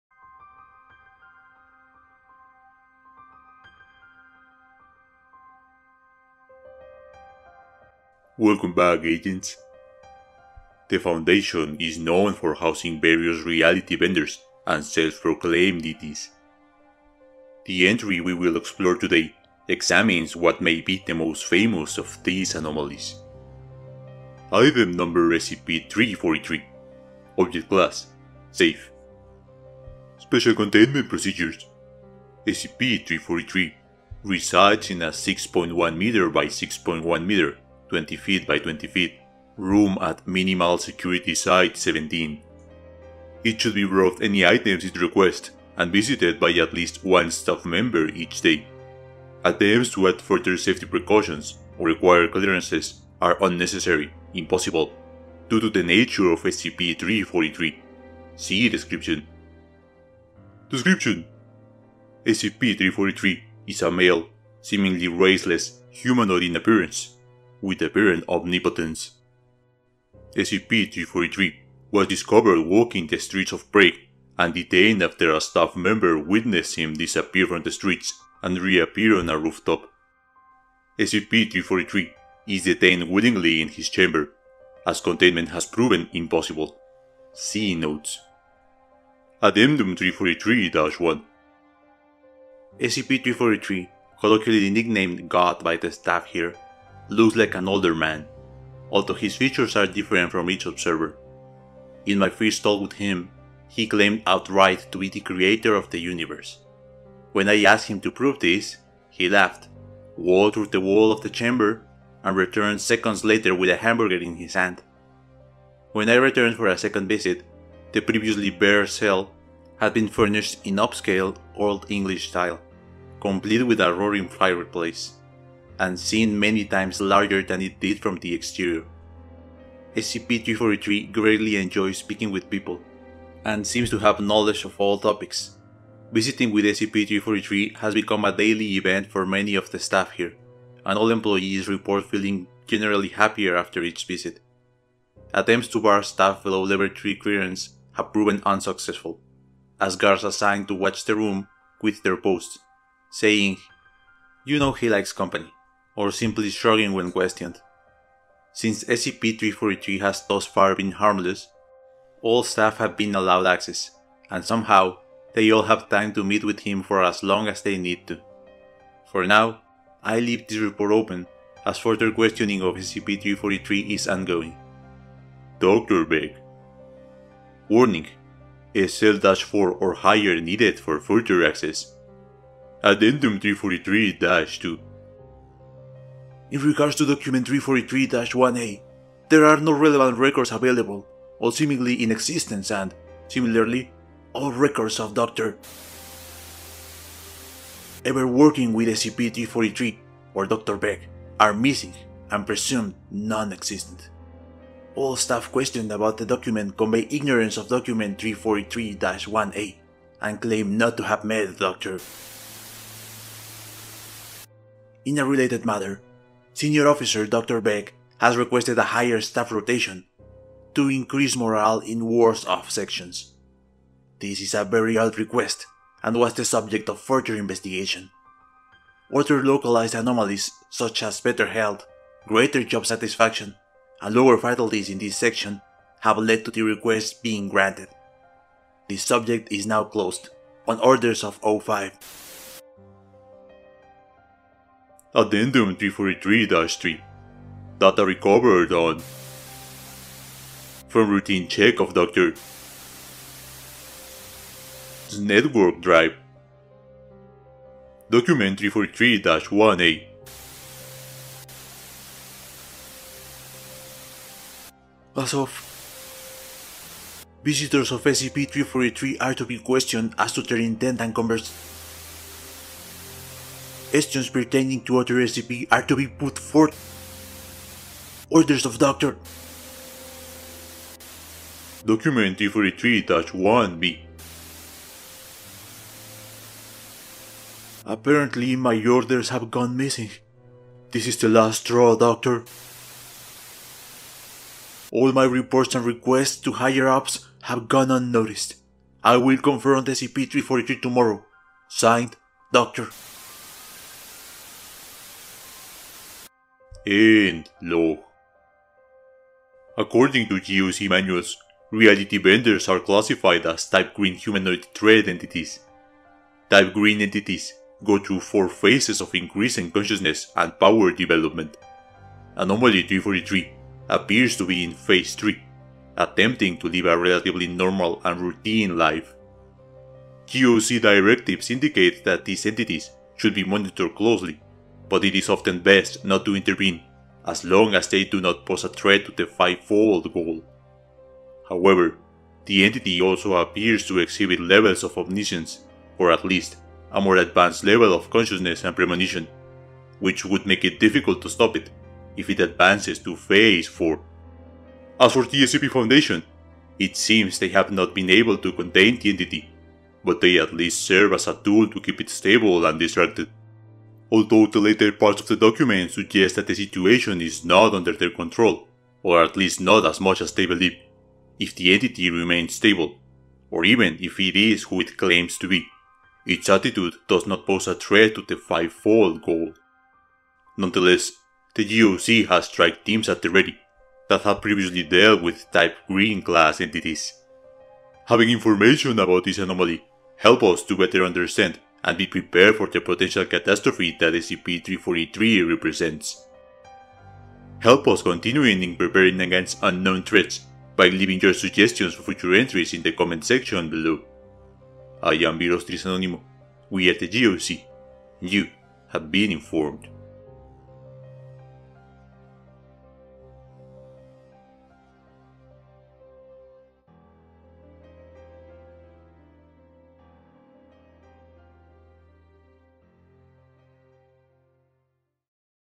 SCP-343 | “God”
The self-proclaimed "Creator of the Universe" appears to have taken up residence in one of the Foundation's containment sites. Is He really an all-powerful deity? Or is he another reality manipulator with delusions of grandeur?

MUSIC ATTRIBUTION

Art Credit:

Por: BlueWolfArtista.